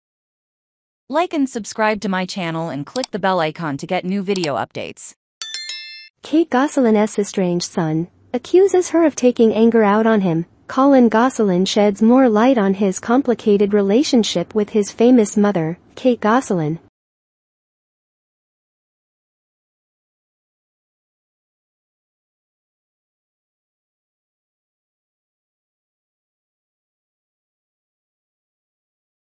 KATE GOSSELINS ESTRANGED SON ACCUSES HER OF TAKING ANGER OUT ON HIM
Collin Gosselin sheds more light on his complicated relationship with his famous mother. Kate Gosselin.